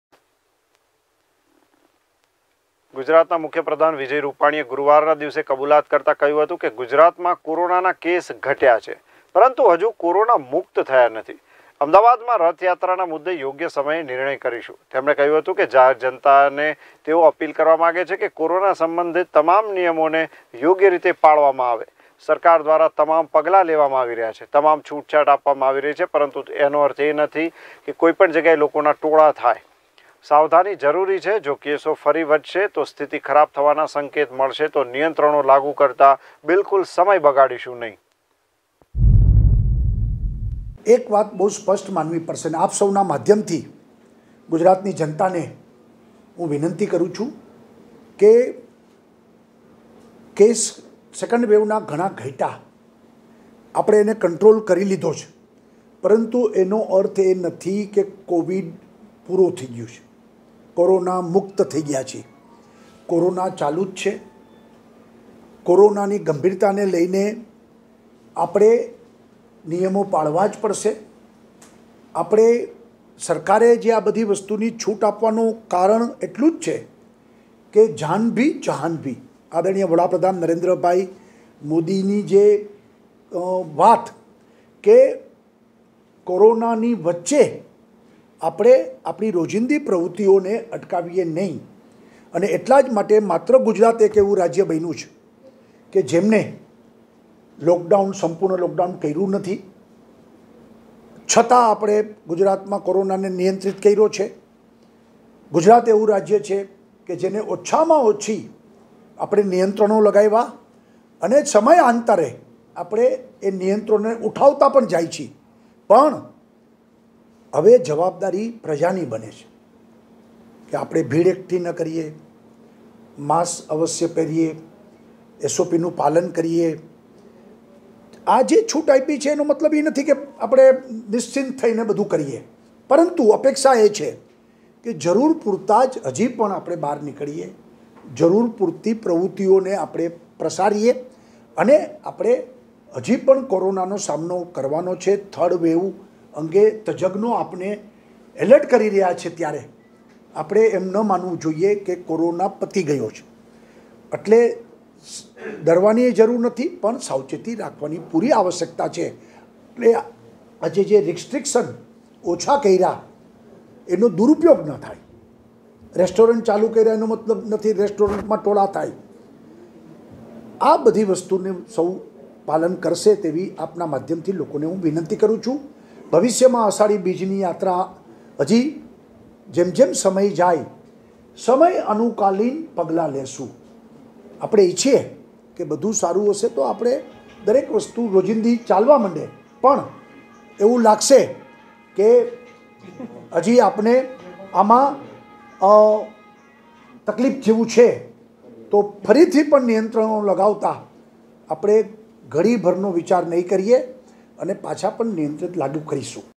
News | ગુજરાતમાં કેસ ઘટ્યા છે પરંતુ કોરોનામુક્ત થયા નથી : સીએમ રૂપાણી | VR LIVE
We are available 24/7 for you on this platform:

GTPL | Paytm | Daily Hunt | Joy TV | Rock TV | Harsh Communication

Anchor - મહિમા,ધ્વની,કુમાર,રોકી  |  Copy Editor - ગોપાલ

Edited By - મેહુલ |  Uploaded By - વરુણ